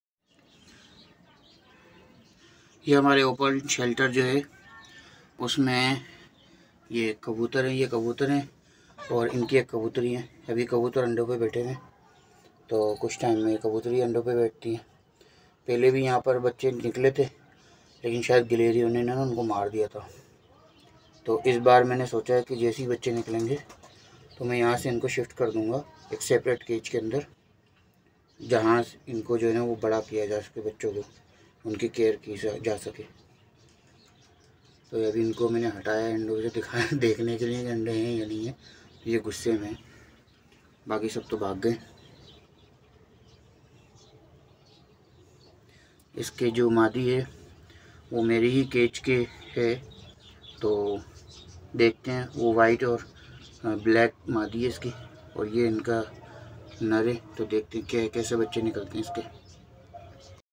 Kaboter andon par beth gaya | pigeon hatch eggs | pigeons shulter for breeding | Jahangir Collection
Mera pigeons shulter ma ek kabooter ka pair na anda dedia han or eggs hatch shuro kardi ha.